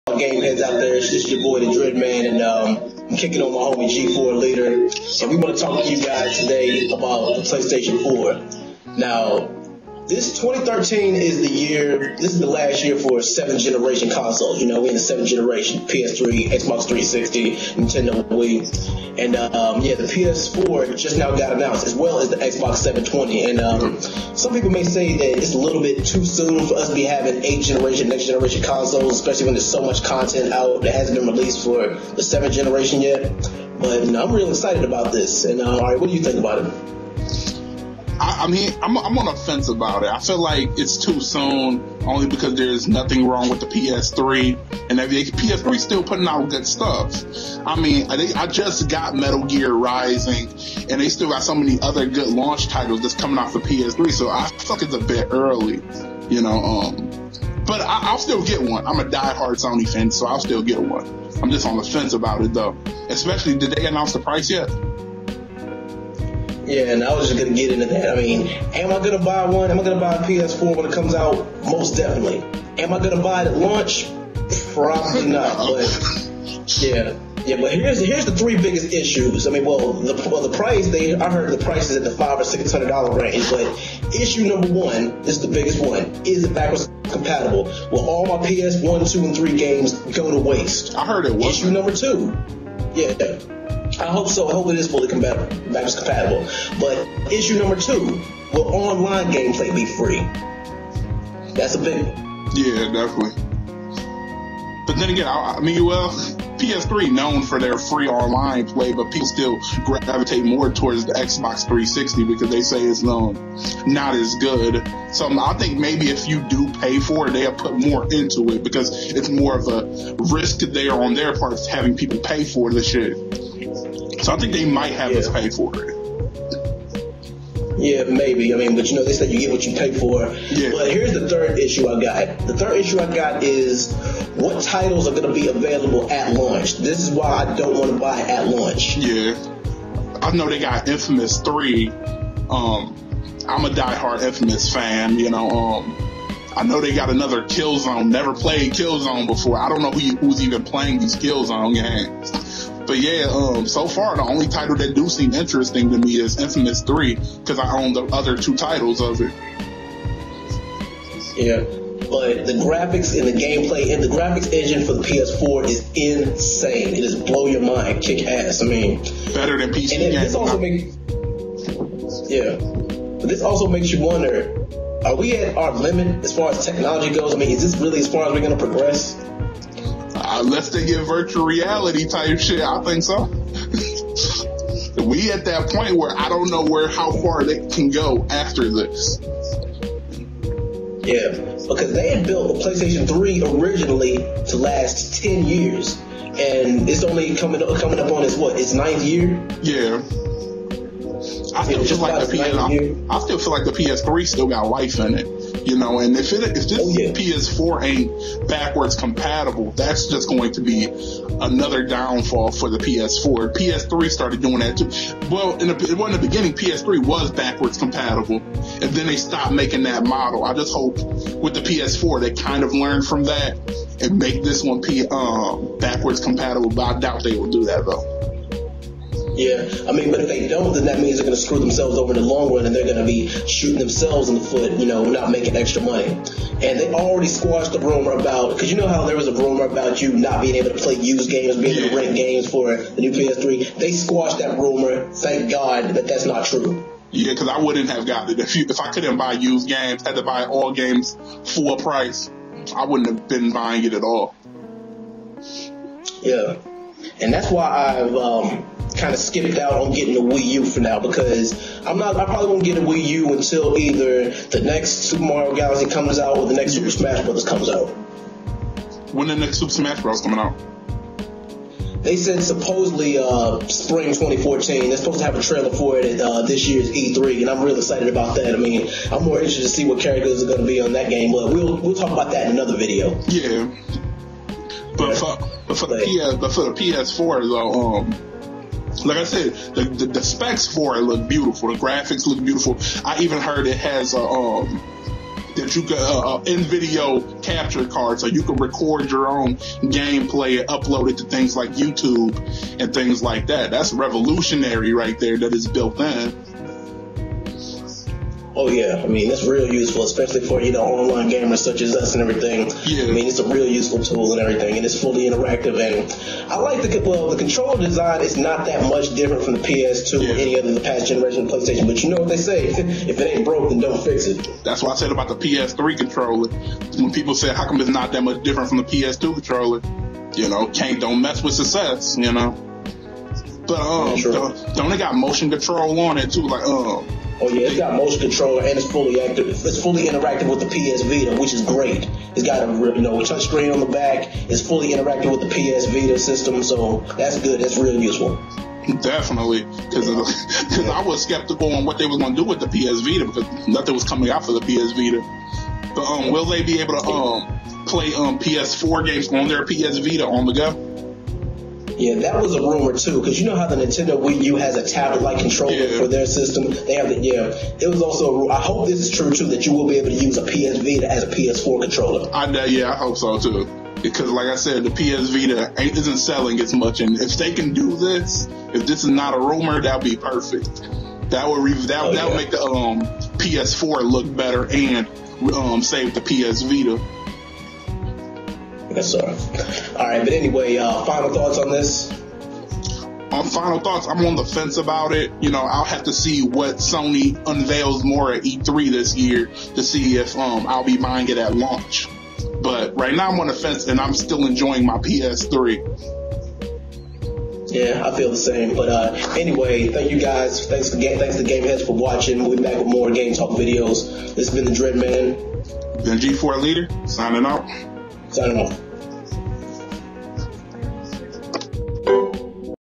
Sony PS4 Launch Discussion
Me and the homie dreadman sit and talk about sonys unveiling of the ps4. and its titles and price point at launch. Is it too early for the ps4? and will it be worth getting when the ps3 still doing its job?